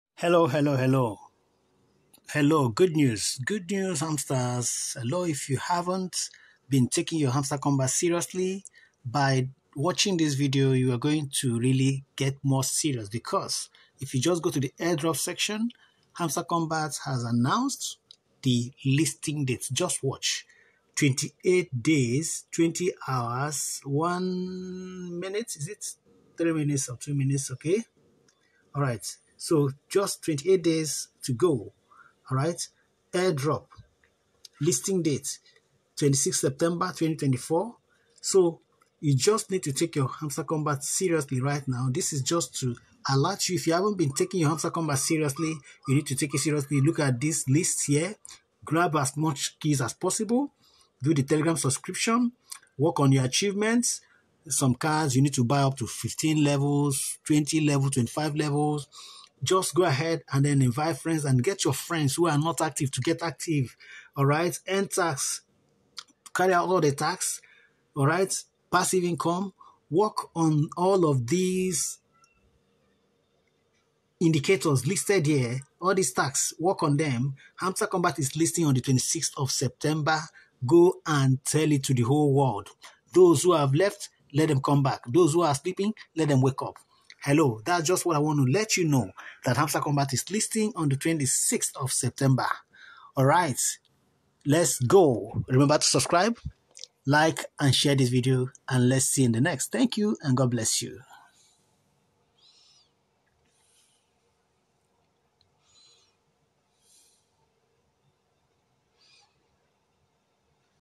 Hamster Kombat Announces Listing Date | List Tasks the Take Very Seriously
You can join Hamster Kombat with the link 😀 here: Link 1